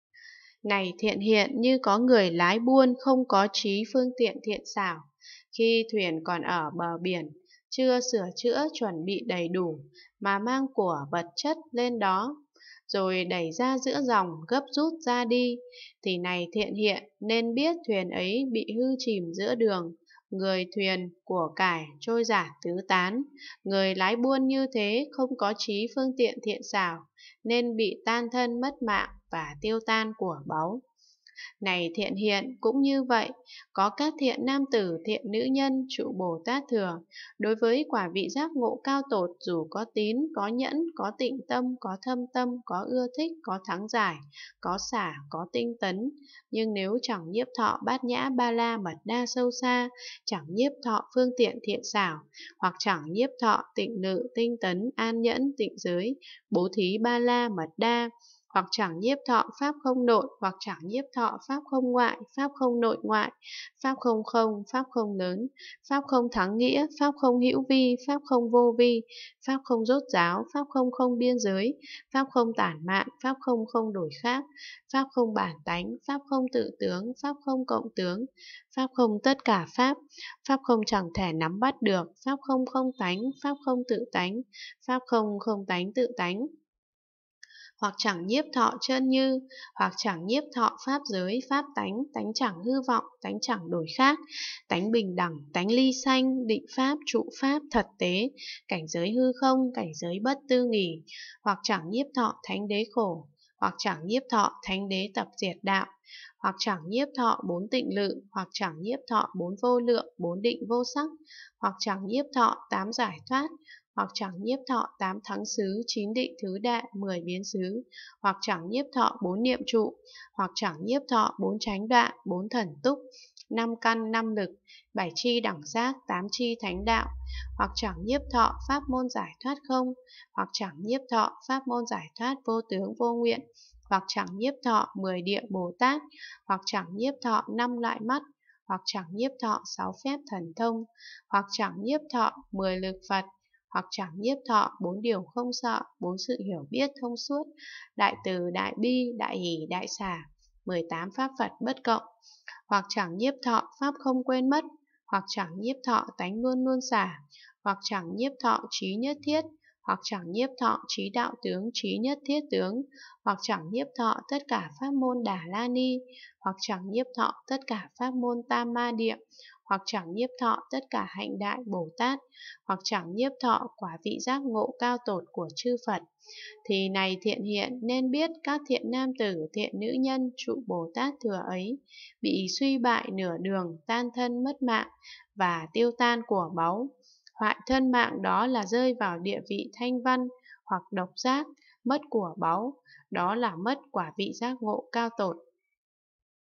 Quyển 312: Kinh đại bát nhã ba la mật đa - Phẩm chúng dụ (rất hay) (312/600) (8)
Phẩm 44: Phẩm chúng dụ
Hán dịch: Pháp sư Huyền Trang
Việt dịch: Hoà thượng Trí Nghiêm
Giảo chính: Hoà thượng Quảng Độ